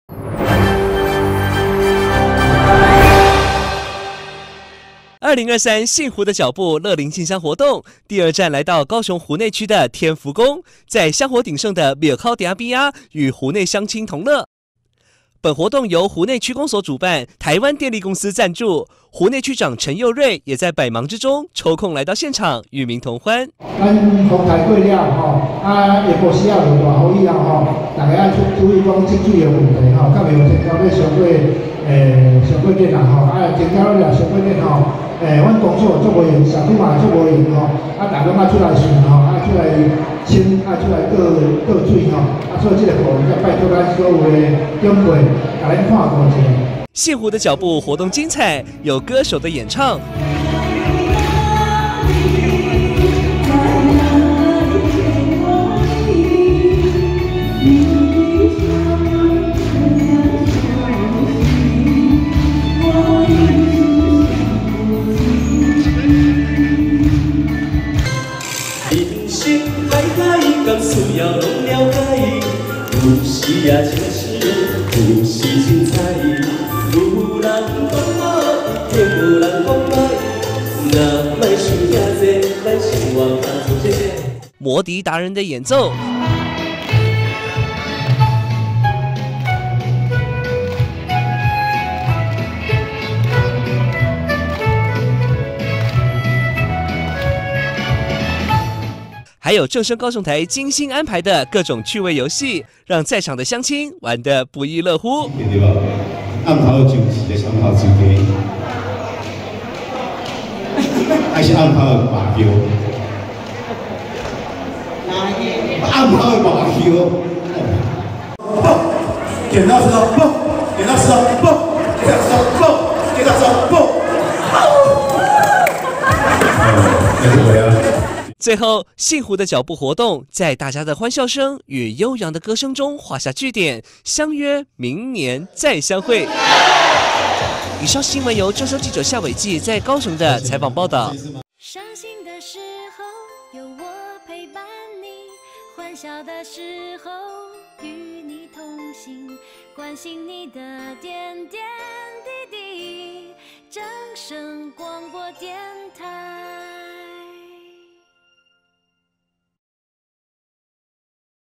【CSNEWS】幸湖的腳步 天福宮進鄉傳愛/CS看正聲
2023幸湖的腳步樂齡進鄉活動第二場來了!!!
 來到高雄湖內的天福宮與民同樂
創造最美好的回憶